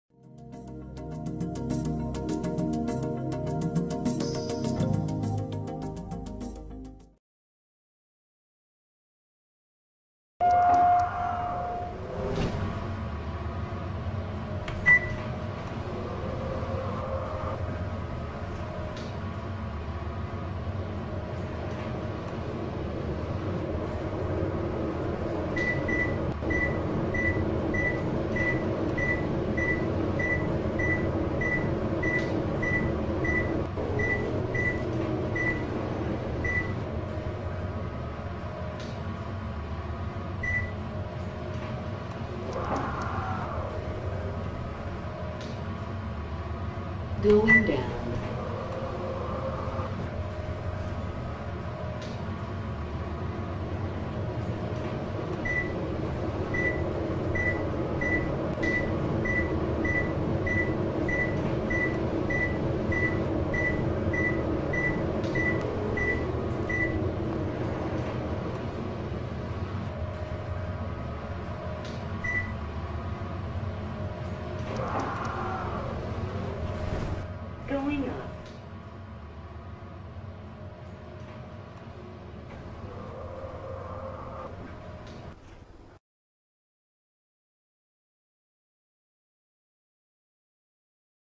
Tour of the Elevators @ Simple - 8 Lifts Building
This is the elevator tour of the Simple - 8 Lifts Building, a 20-floor Simple Building that has a total of 8 elevators.